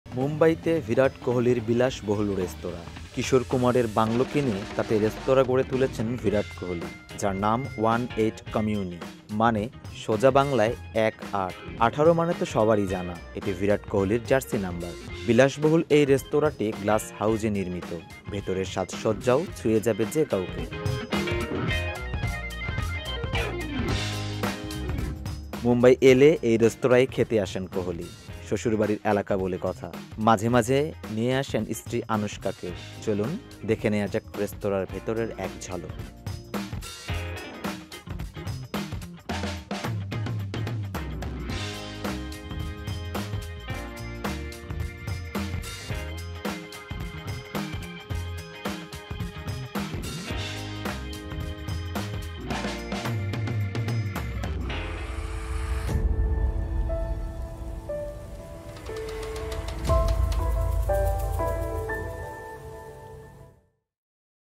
একনজরে মুম্বাইতে বিরাট কোহলির বিলাশবহুল রেস্তরাঁ
কিশোর কুমারের বাংলো কিনে রেস্তোরাঁ গড়ে তুলেছেন বিরাট কোহলি